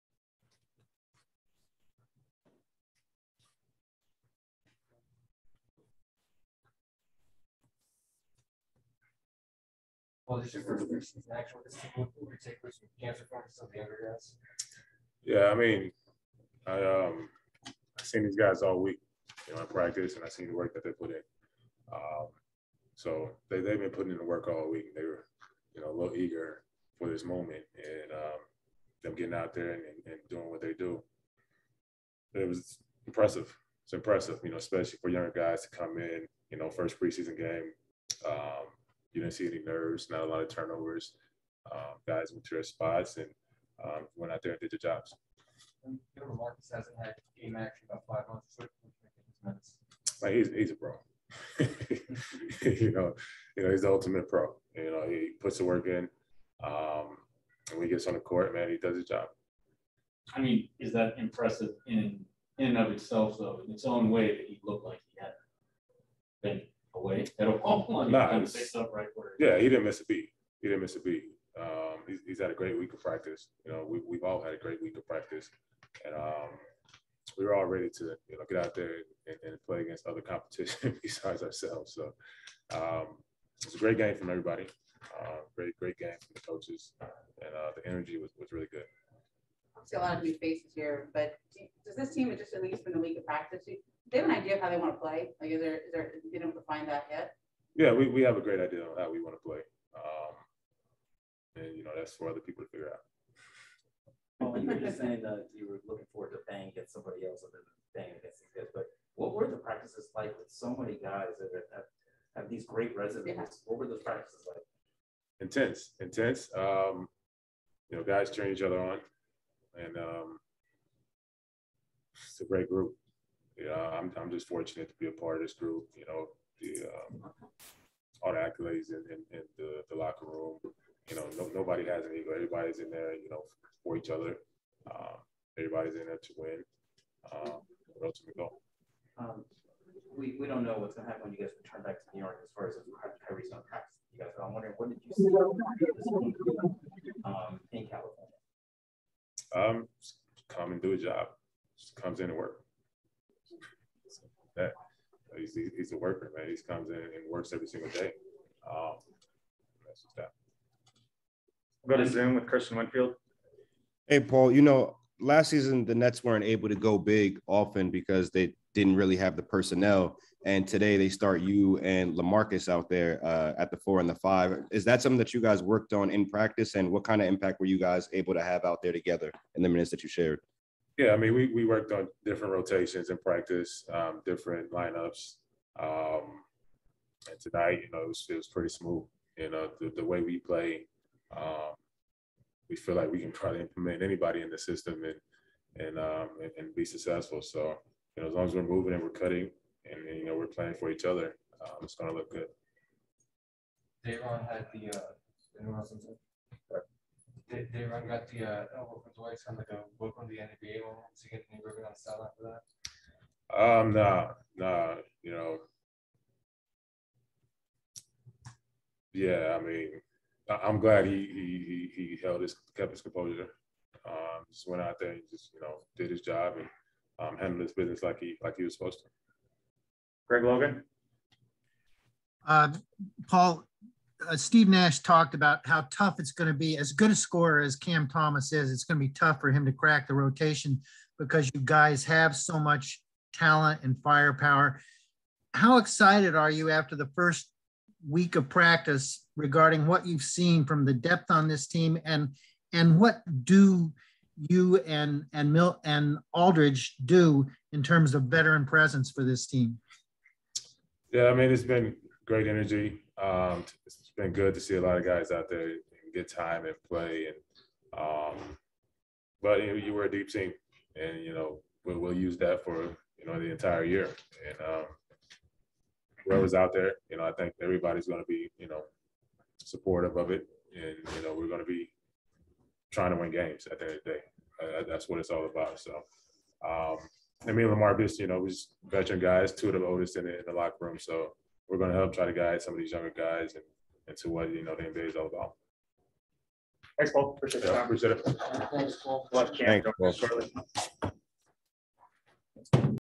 Paul Millsap Postgame Media | Nets vs. Lakers | 10/3/21
Brooklyn Nets forward Paul Millsap speaks to the media after his double-double in the Nets' 123-97 win over the Lakers.